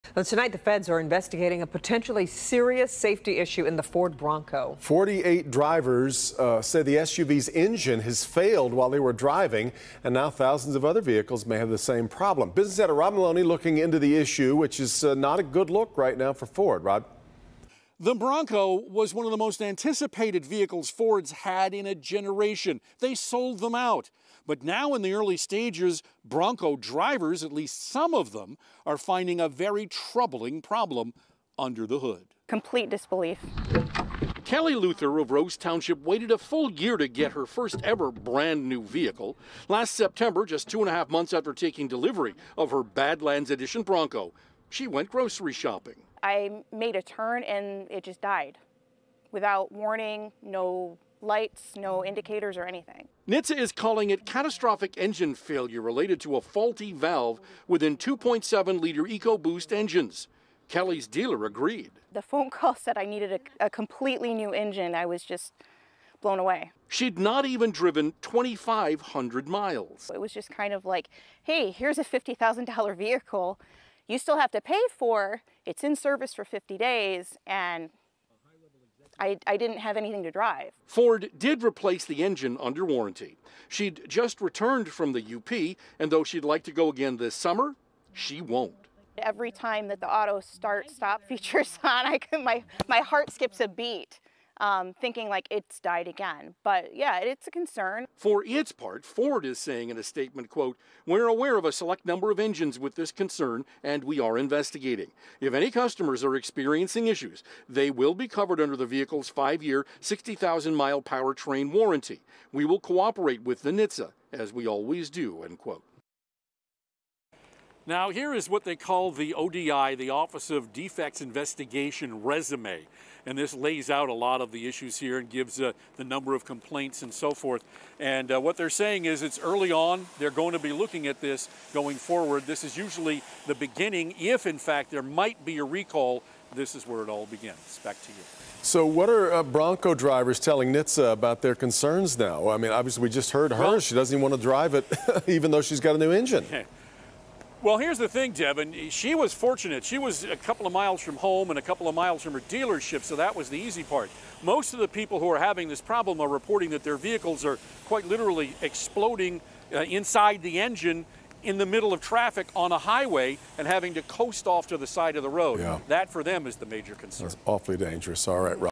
Feds investigate Bronco engine failures
48 drivers say the SUV's engine has failed while they were driving. Now thousands of other vehicles may have the same problem.

‘I made a turn, and it just died without warning’: Feds investigate Ford Bronco engine failures